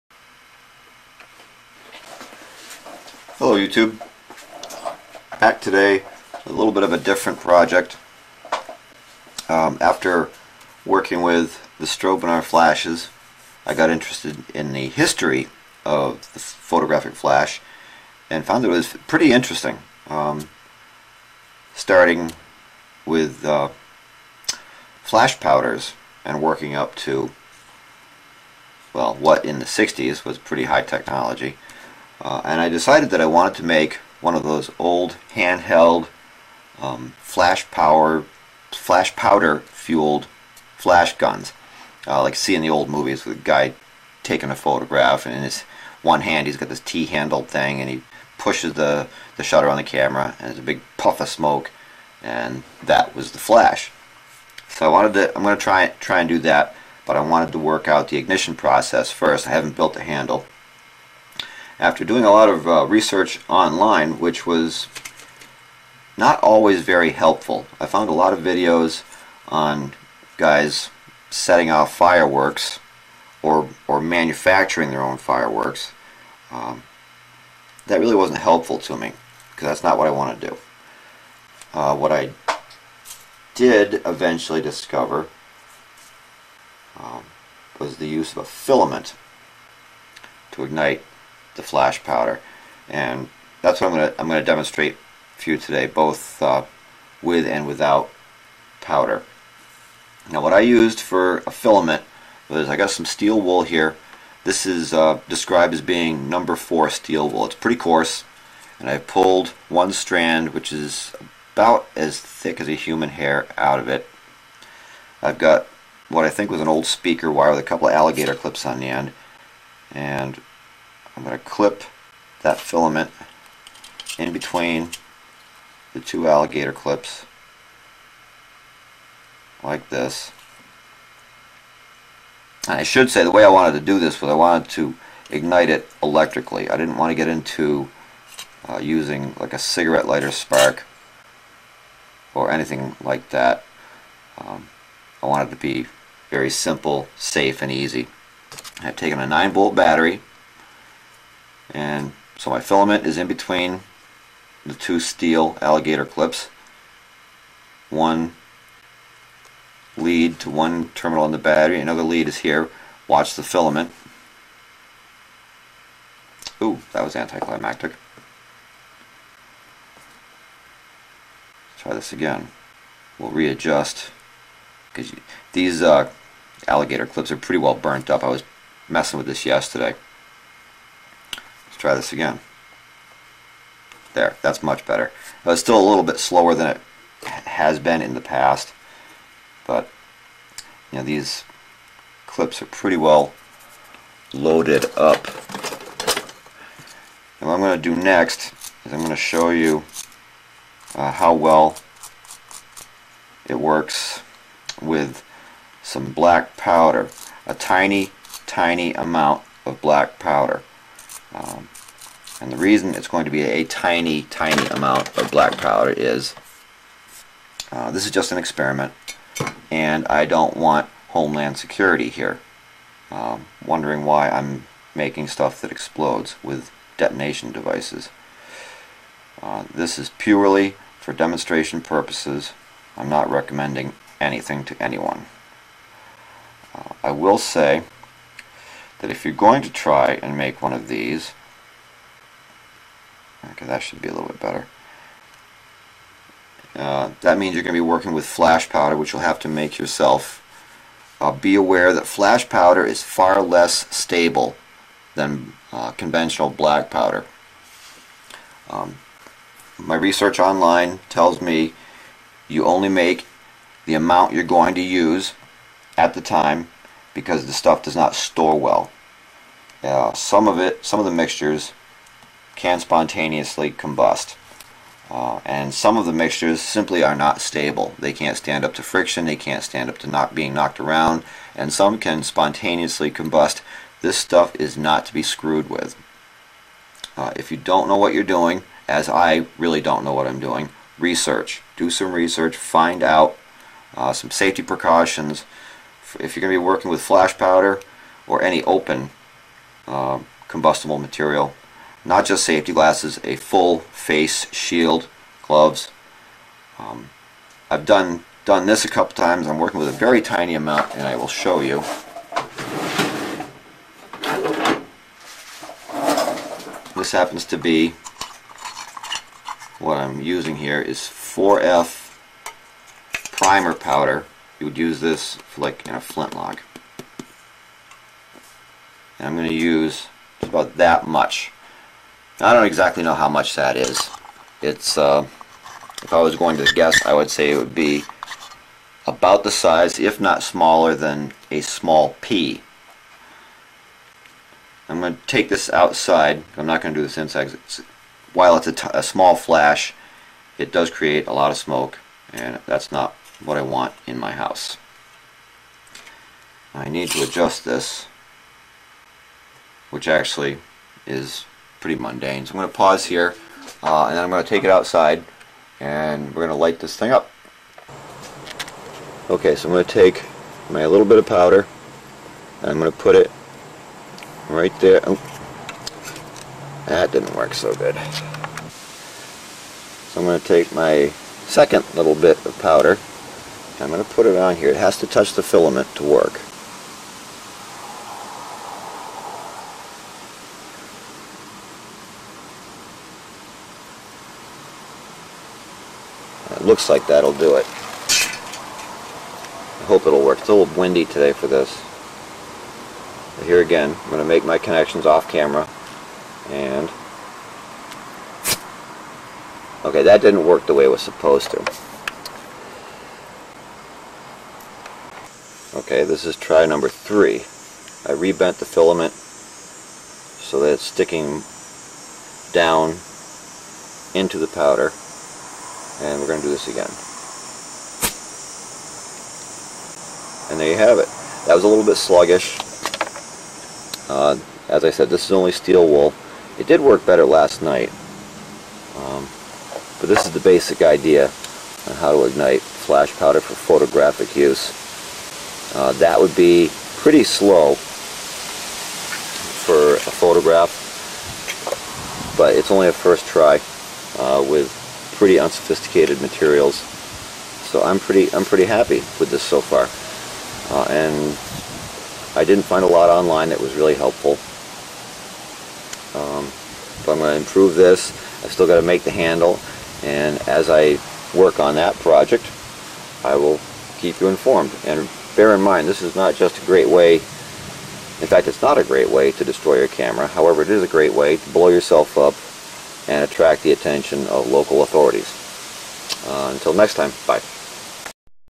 Flash powder flash gun for photography
This video is the first step in making an early photographers flash gun fueled with flash powder. Its a little incomplete but is only intended to demonstrate a simple way to ignite flash powder. This is experiment #1. The handle is yet to be made. below are a pic and a link to clarify my intentions. Remember, flash powder is NOT a toy!! Research tells me that stuff is at best semi-unstable. Use caution PERMANENT BLINDNESS AND OTHER INJURIES CAN OCCUR IF YOU ARE CARELESS!!